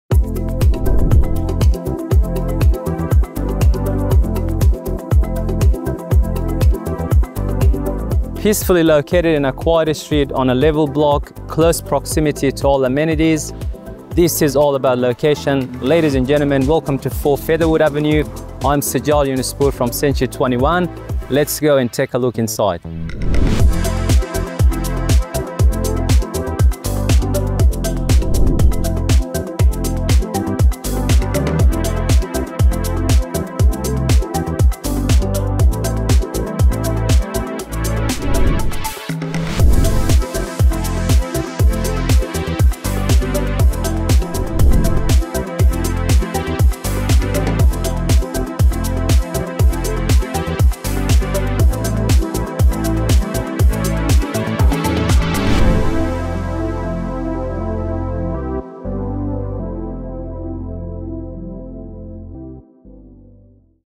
4 Featherwood Avenue, Cherrybrook | Perfect Location - Zoned for Cths
Peacefully positioned on a quiet street, this well-presented family home offers an idyllic lifestyle and superb functionality throughout. Set amidst a generous level block and surrounded by established gardens, it presents relaxed and private family living. As you step inside, you're greeted by a welcoming living area to the right, perfect for relaxing or entertaining. The heart of the home features a central kitchen with ample space, flanked by a generous dining area and a versatile rumpus room, ideal for family gatherings or playtime.

Upstairs, you'll find four well-sized bedrooms including the master bedroom with ensuite, providing plenty of space for growing families. Two bathrooms cater to your needs, offering convenience and functionality, conveniently located near to some of the leading schools in the Hills District, Cherrybrook Technology High School and John Purchase Public School with easy access to Cherrybrook Shopping Village, Appletree Shops, Sydney Metro and parks. The bus stop is within walking distance, adding to the convenience of this peaceful locale.

This home is ready for a new owner to bring their vision to life. With some renovations, you can add your personal touch and transform this property into a contemporary haven tailored to your taste.

Seize this unique chance to own a property with endless potential in a desirable Cherrybrook location.

Don't miss out on making 4 Featherwood Avenue your new home.
Contact Sajjad Yunespour today to arrange a viewing!

Features:
+ Peacefully positioned family home on a quiet street
+ Generous level block and surrounded by established gardens
+ 4 well-sized bedrooms - Master bedroom with built-ins & ensuite
+ Formal lounge, generous dining and a versatile rumpus
+ Covered entertaining area overlooking the level grassed yard
+ Central kitchen with gas cooking, dishwasher & ample storage
+ Easy access to Cherrybrook Shopping Village, Appletree Shops, Cherrybrook Metro and local parks
+ An easy 180m walk to local & express city bus stop
+ Includes split system air-conditioning, double auto garage & a single carport
+ Catchment for Cherrybrook Technology High School & John Purchase Public School

Locations features:
+ 850m to Appletree Drive Shops
+ 1.4km to Greenway Park
+ 1.5km to John Purchase Public School
+ 1.7km to Cherrybrook Technology High School
+ 1.7km to Cherrybrook Village Shopping Centre
+ 2.1km to Carlile Swimming Cherrybrook
+ 6.1km to Castle Towers Shopping Centre

Proudly Marketed by Sajjad Yunespour
CENTURY 21 Joseph Tan Real Estate